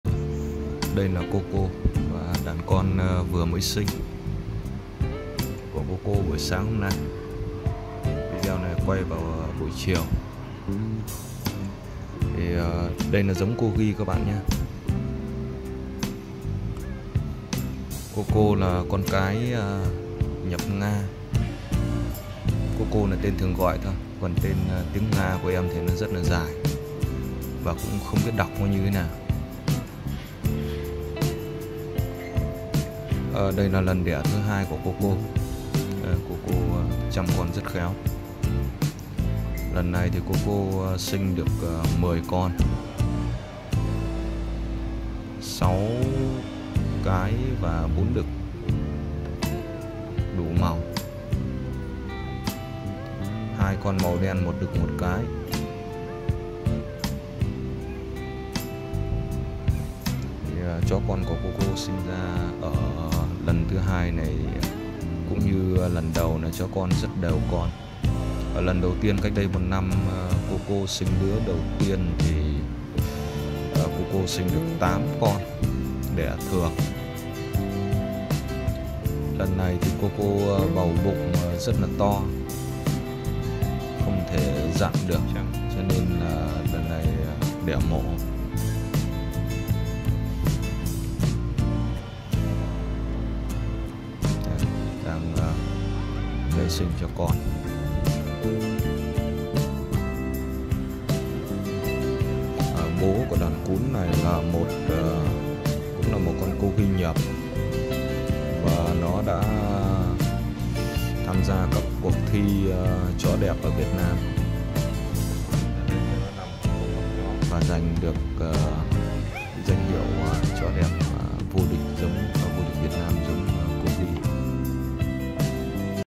Đàn chó Corgi sơ sinh bố mẹ nhập yêu mê mẩn bao người của Vườn Bách Chó Gervi Adorable Corgi Puppy
Đàn chó Corgi sơ sinh của cặp bố mẹ ChóCorgi nhập và Coco bà mẹ đảm đang của đàn Adorable Corgi Puppy siêu đáng yêu của Vườn Bách Chó Gervi. Coco là chó Corgi nhập Nga của trại mang khuôn mặt tuyệt đẹp và tính cách đáng yêu nữ tính. Đàn chó con là con của Coco và chó đực nhập đã đạt danh hiệu chó đẹp quốc gia của Việt Nam giống Corgi năm 2018. Các bạn yêu chó đừng quên để lại bình luận và chia sẻ video nhé.